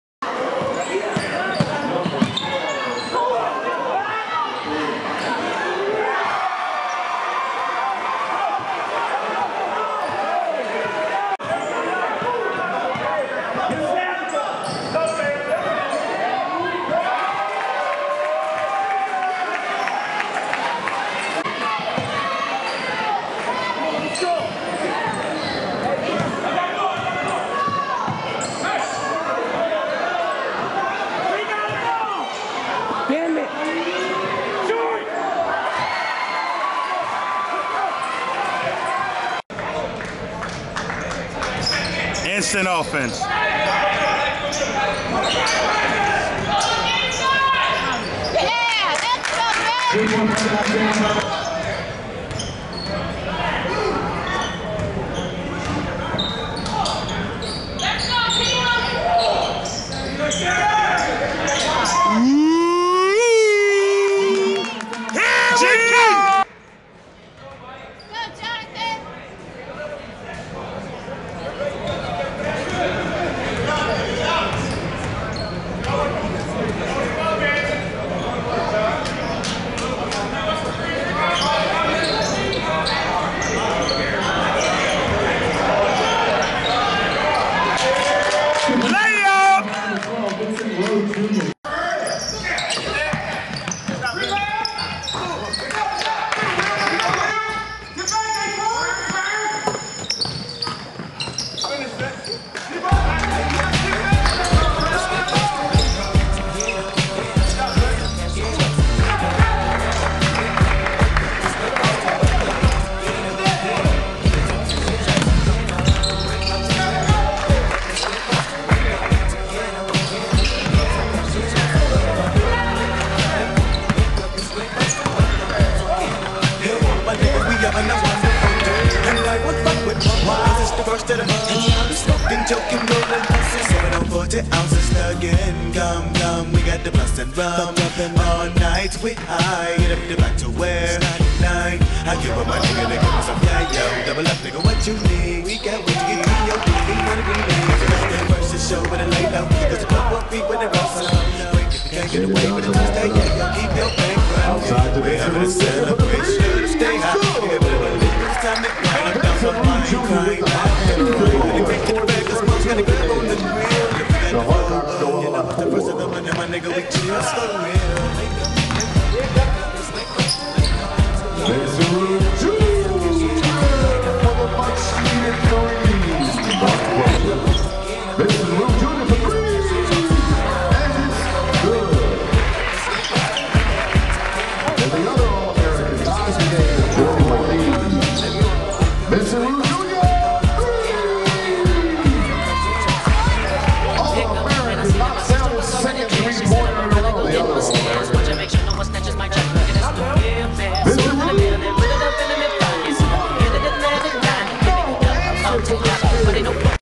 Vincent Roux Highlights
Vince ROux shows Scouts why he is worthy as a dead on shooter. Its not a coincidence he is a career 42% 3 point shooter folks